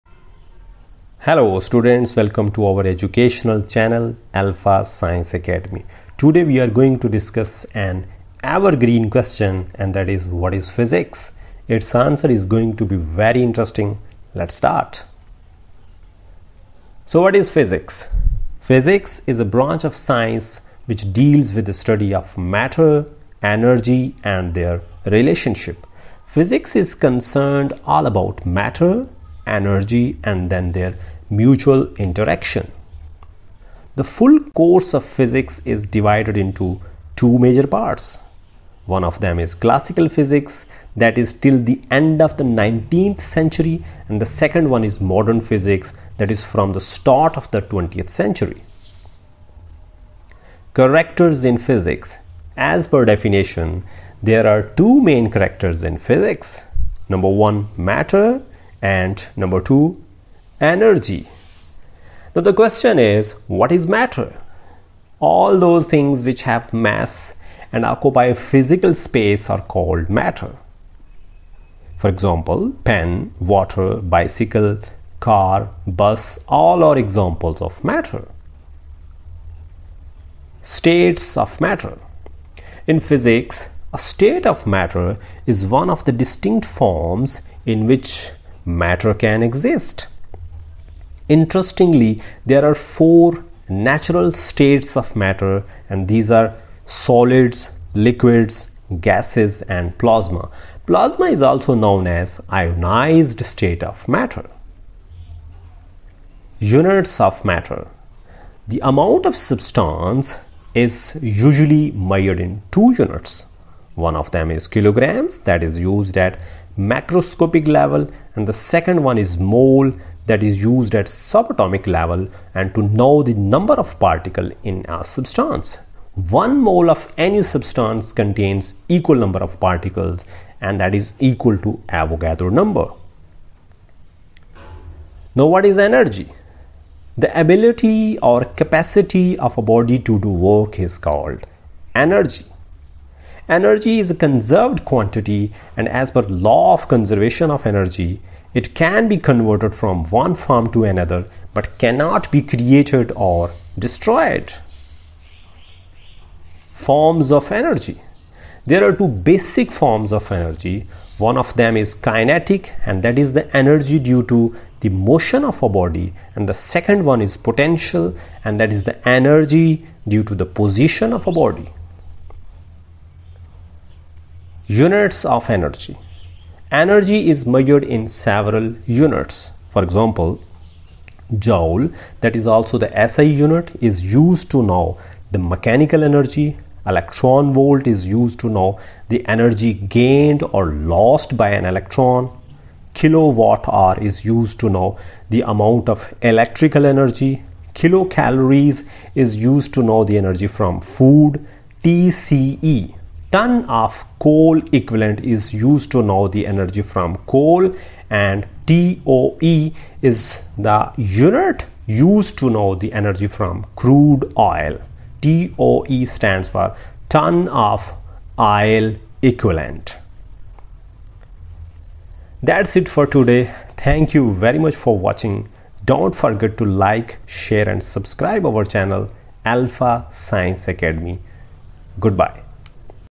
What is Physics? (in English)
in this short video clip that is very crunchy video we have discussed the following things in detail:
1. What is Physics, we have given its definition and then we have explained that why the full course of physics is divided into two major parts the one is classical physics and the second one is modern physics, classical physics belongs to an era that ends at the last of 19th century and modern physics belongs to an era that starts from the beginning of 20th century.
2. then we have define the characters in physics and those are matter and energy and then we have given a definition of matter that is what is matter and matter is all those things which have physical space then we have discribed different states of matter and those are solid liquid gas and plasma and then we have given some units of matter and the second one is the energy what is it.
3. Energy is the ability or capacity of a body to work and then as per the law of conservation of energy we have define that energy is a conserved quantity and  can be converted from one form to another and then different basic forms of energy that are kinetic and potential energy are given and at the video we have discussed different units of energy and that are joules, electron volt, kilowatt hour, kilo calories tce and toe.

regards and prayers